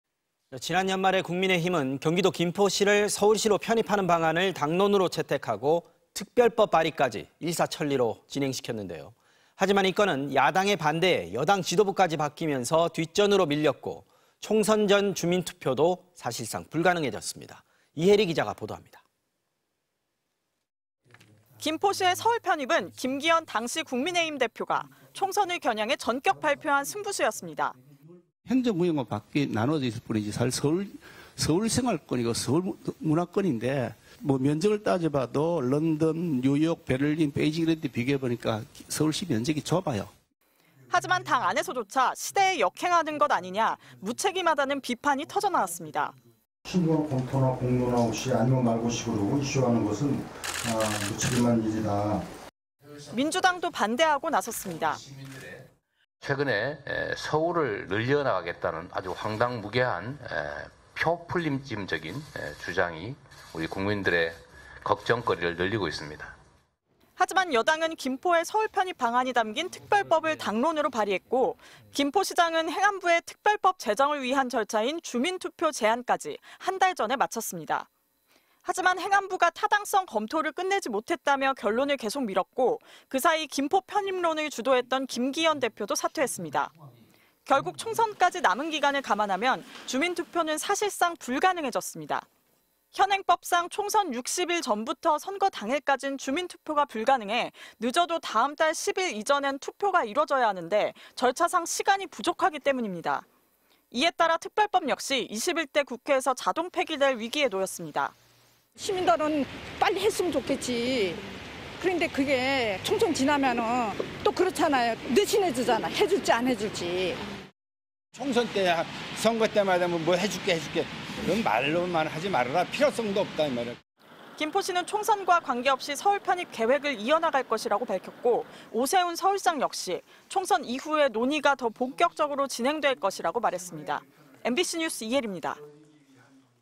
'김포 서울 편입' 주민투표 총선 전 불발‥특별법 폐기 위기 (2024.01.17/뉴스데스크/MBC)
지난 연말에 국민의힘은 경기도 김포시를 서울시로 편입하는 방안을
당론으로 채택하고 특별법 발의까지 일사천리로 진행시켰는데요.
하지만 이 건은 야당의 반대에, 여당 지도부까지 바뀌면서
뒷전으로 밀렸고 총선 전 주민투표도 사실상 불가능해졌습니다.